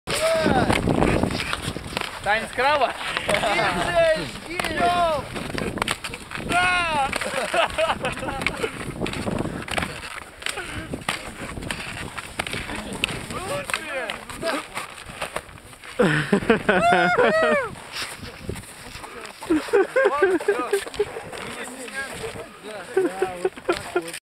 Арктическая чечетка
Арктическая чечетка в исполнении участников молодежной экспедиции "На лыжах - к Северному полюсу!" (апрель 2011 года)